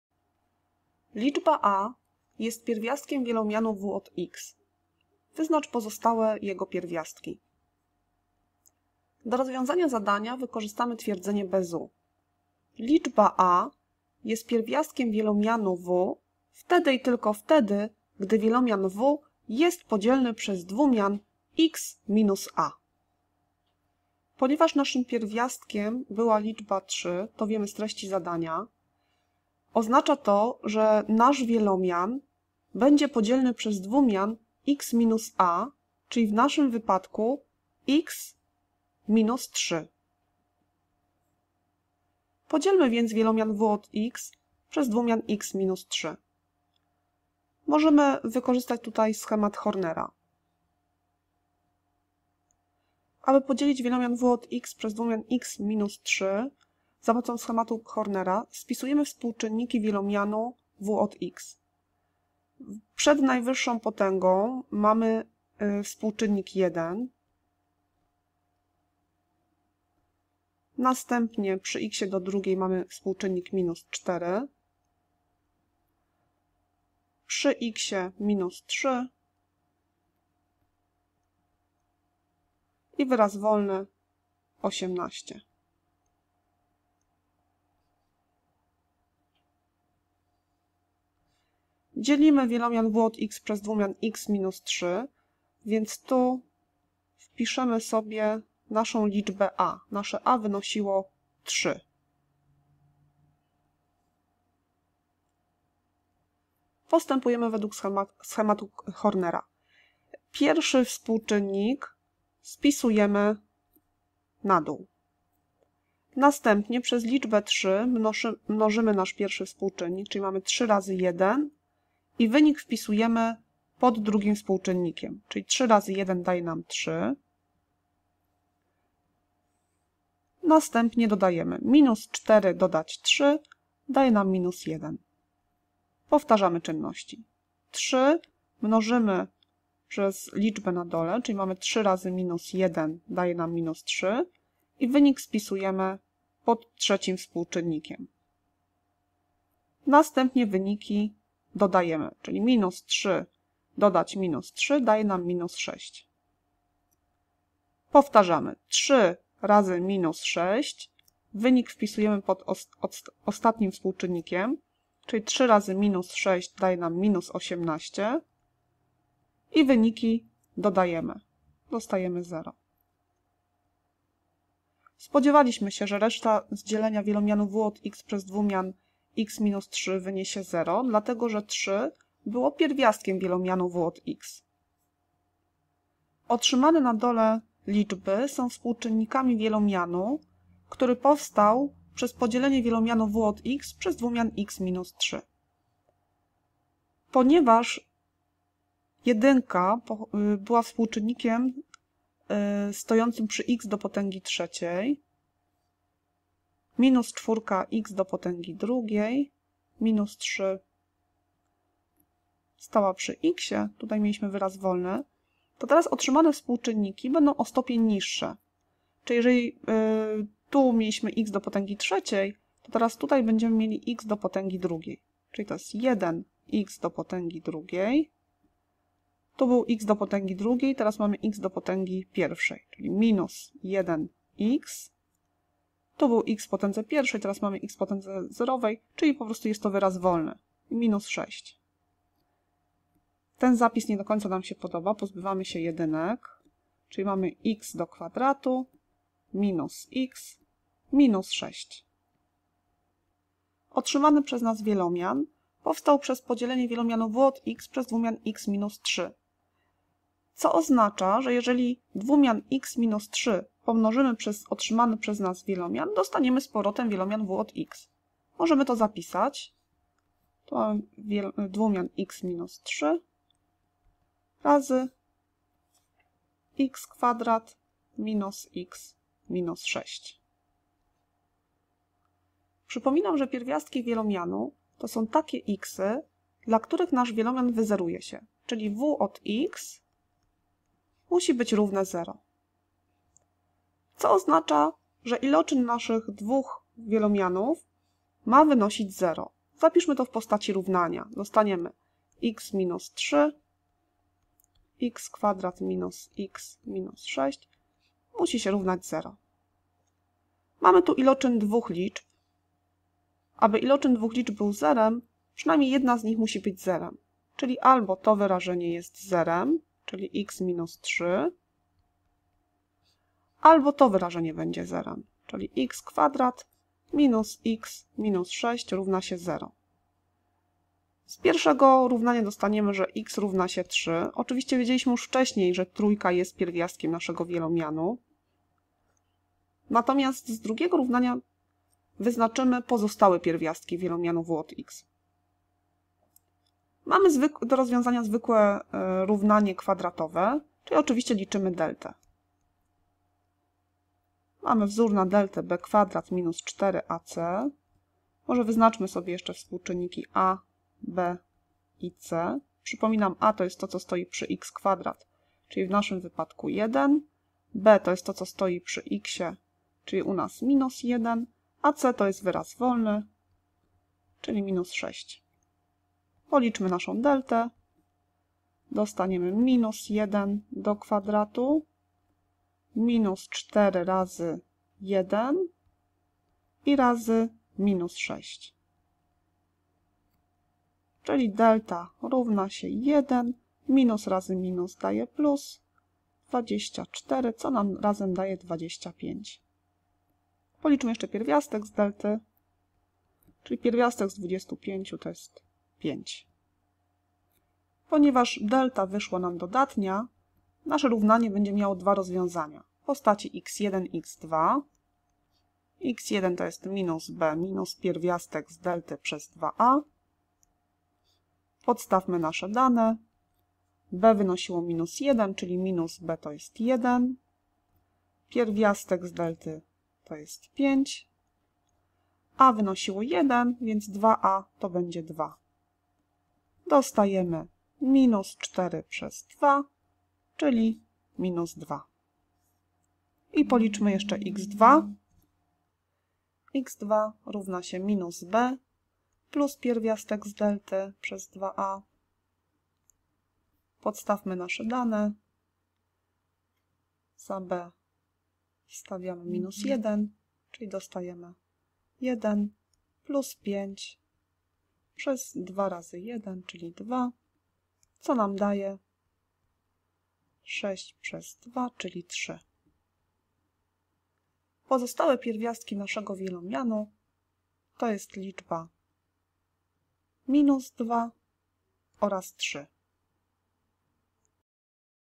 Twierdzenie Bezouta
Liczba a jest pierwiastkiem wielomianu w. Wyznacz jego pozostałe pierwiastki.

Film powstał jako pomoc do kart pracy, które znajdują się na stronie w zakładce: MATMA: WIELOMIANY: TWIERDZENIE BEZOUTA